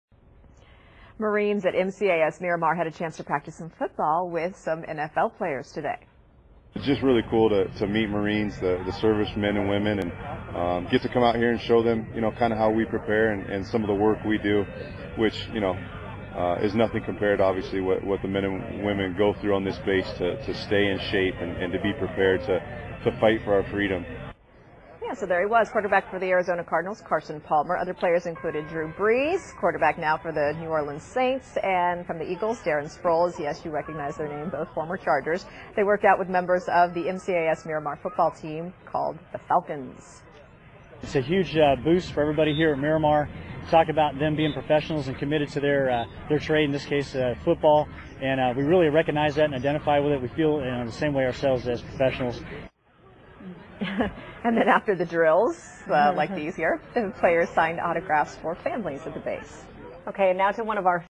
KNSD- NBC News at 4 coverage of NFL event aboard MCAS Miramar
NBC News at 4 coverage of NFL event aboard Marine Corps Air Station Miramar,Calif., July 15. The event featured Drew Brees, Darren Sproles and Carson Palmer.